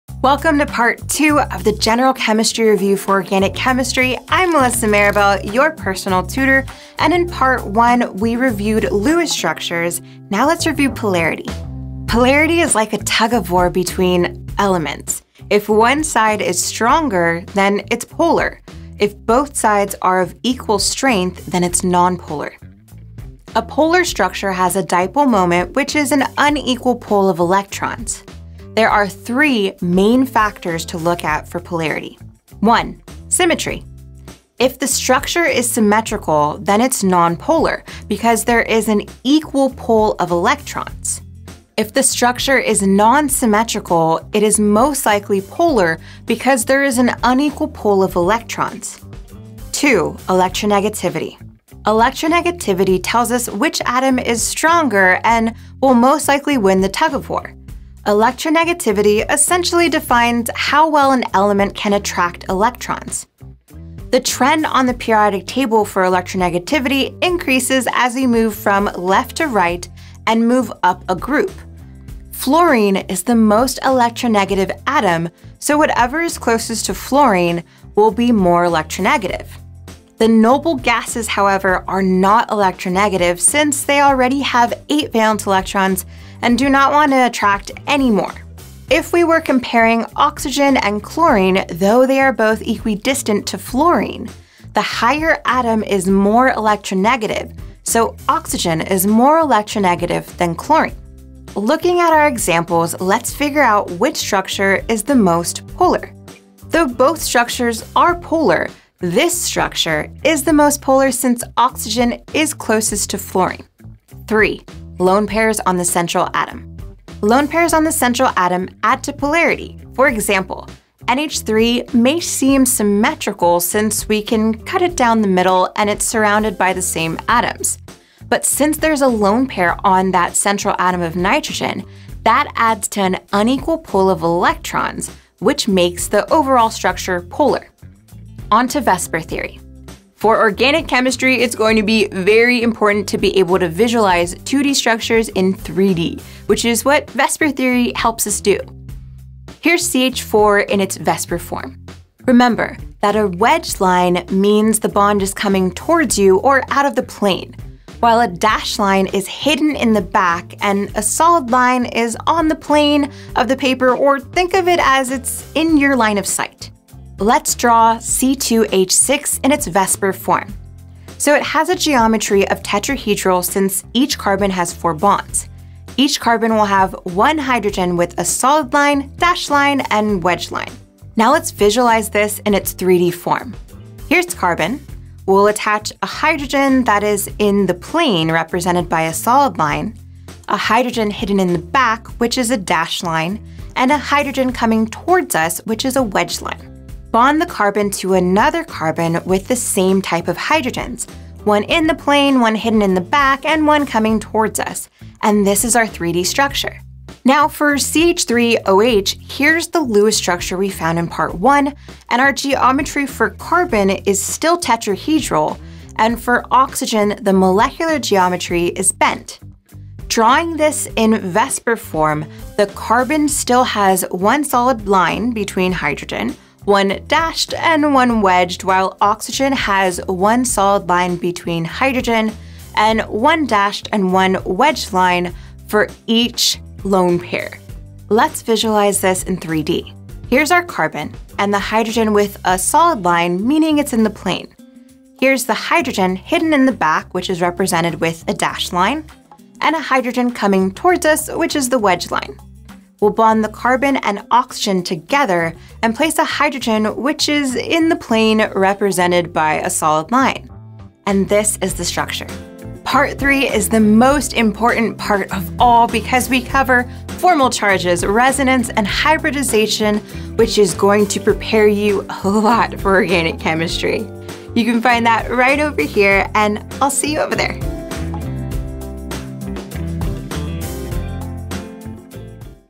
General Chemistry Review for Organic Chemistry Part 2
With this video, I will refresh your memory on polarity and VSEPR theory. I have 3 easy factors to check for polarity so you never question if a molecule is polar or nonpolar. You will also get a detailed review on VSEPR theory, where we'll even see the 3D structure of common ochem molecules. Let's get you ready to pass Organic Chemistry!